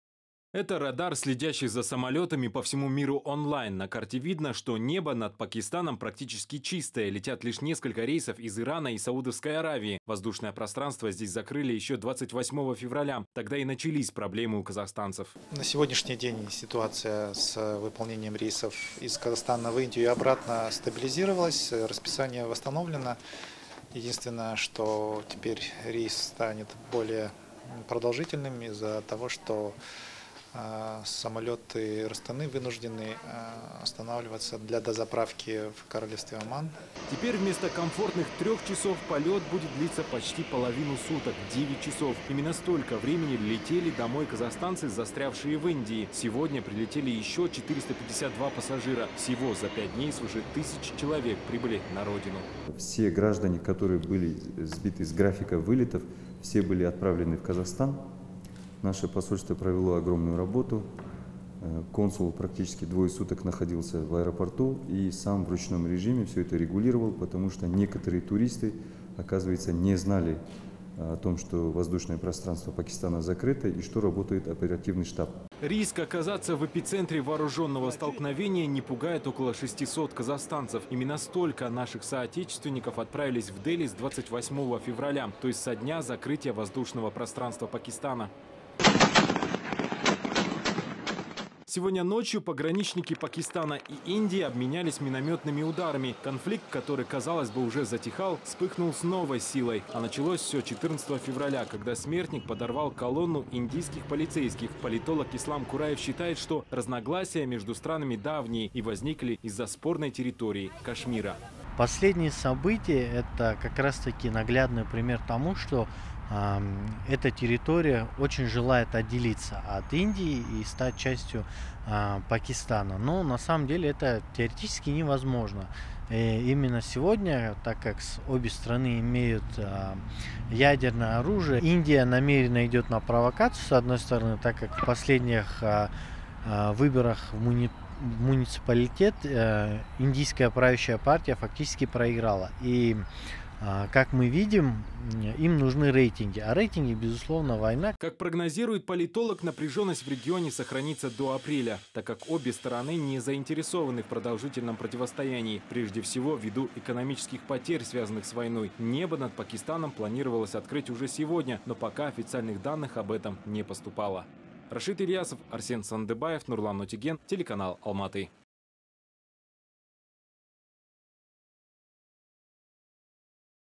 Казахстанцы, застрявшие в Индии, вернулись домой (04.03.19)
Только сегодня на родину прибыли 452 пассажира. Впрочем, наблюдается и обратная ситуация - несмотря на продолжающийся конфликт между Индией и Пакистаном, более 600 казахстанцев отправились в Нью-Дели. Не грозит ли им опасность.